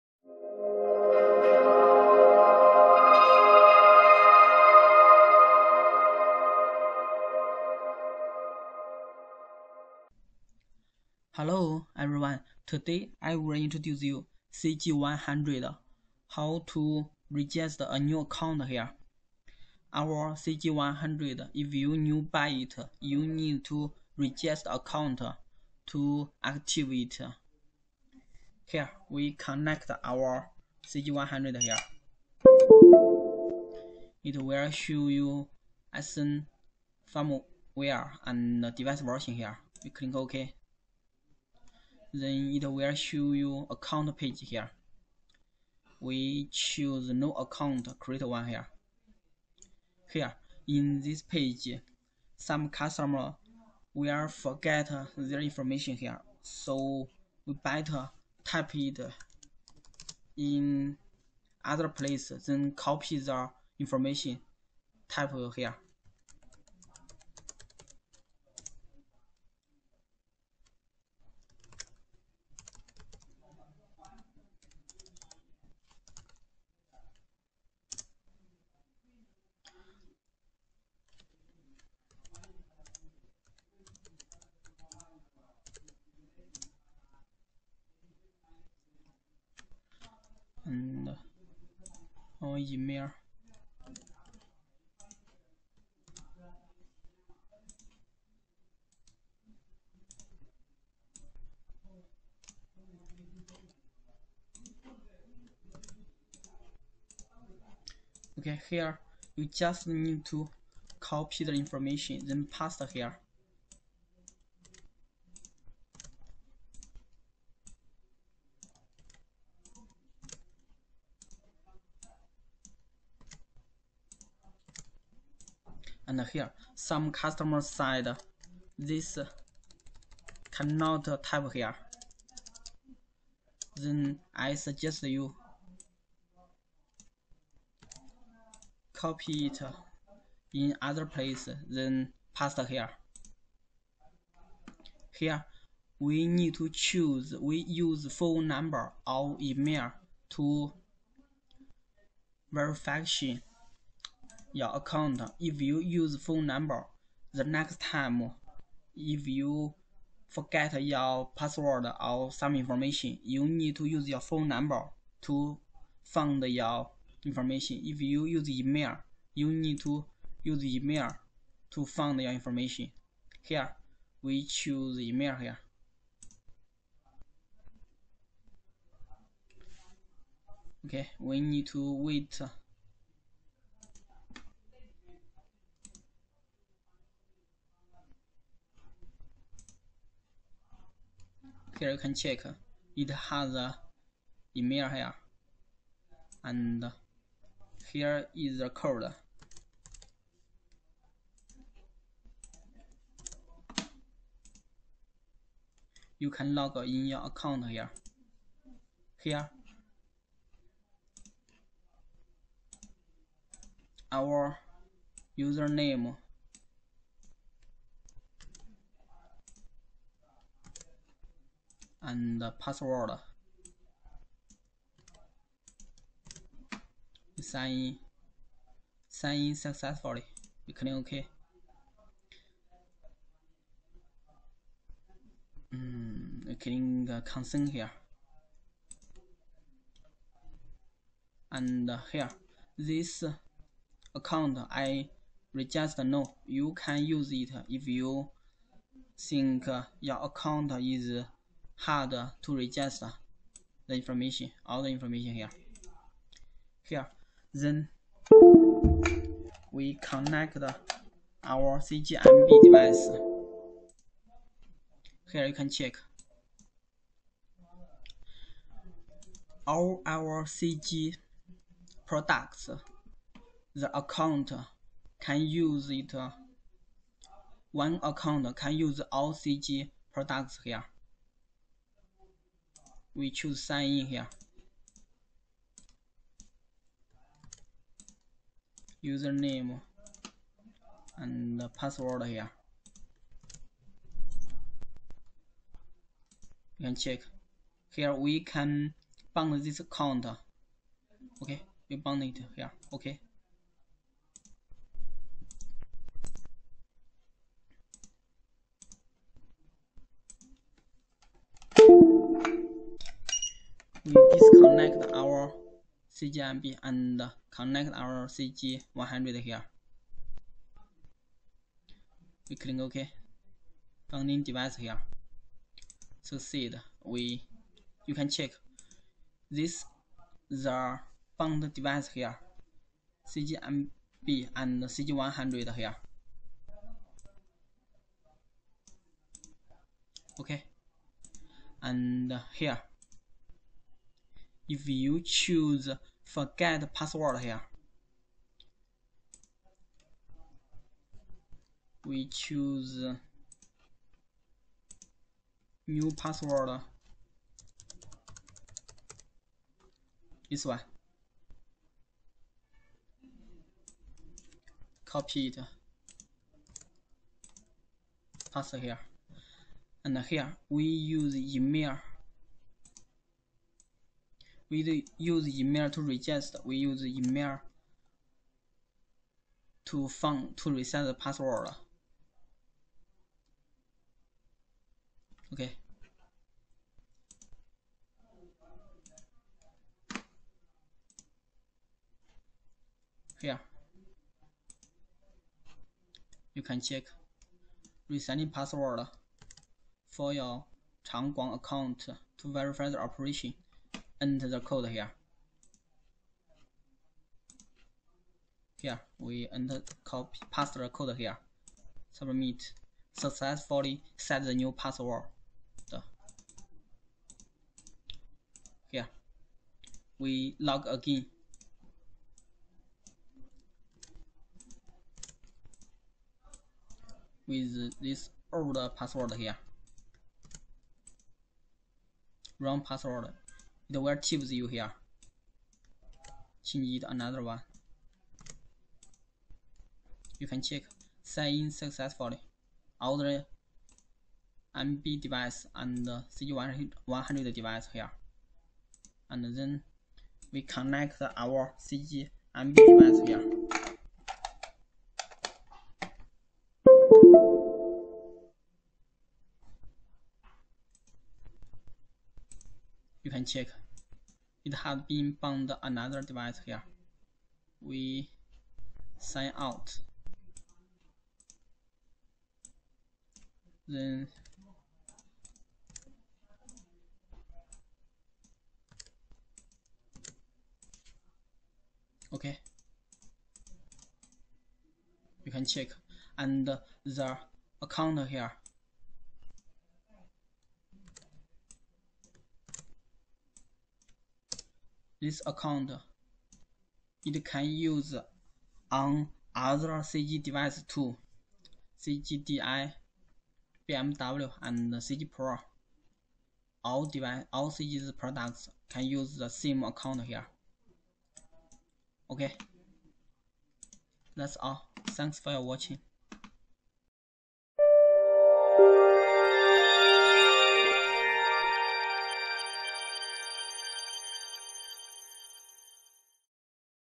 CG100~How to register account and find password
1 All CG's produect can use same account to sign in
2 You need to register a new account to activate CG100 ,if your CG100 are new buy
3 Some customers said it can not type "@" . we can copy it from the other place
4 We choose phone number to verify ,then if your password lost ,you can find it by phone number only  .Same as email .
5 if you dont remember your use name ,contact CG techsupport ,send your register way (number ).They will help you to solve it